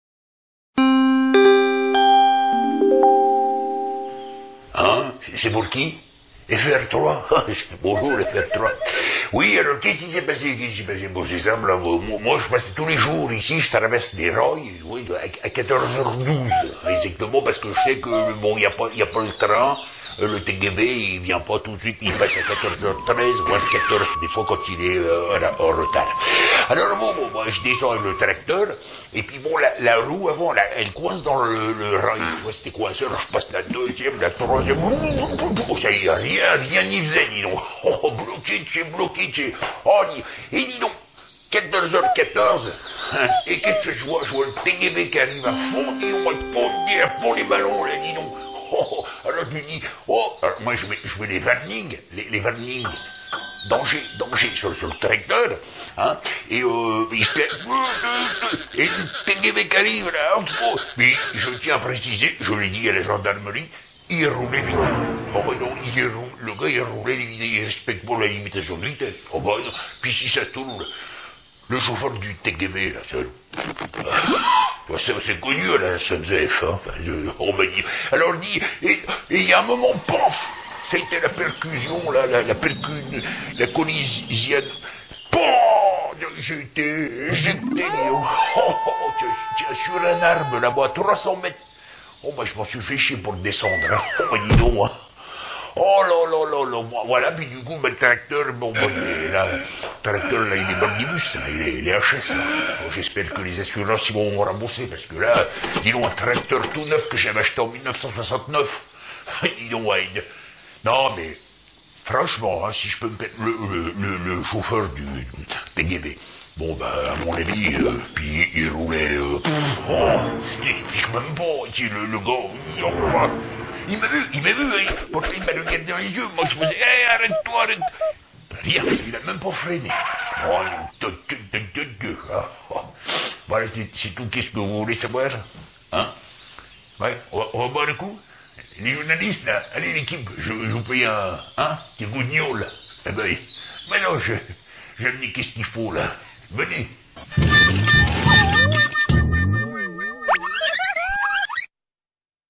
L'accident
Témoigne d'un miraculé, suite a un accident hors du commun.

Merci d'avance  ;-)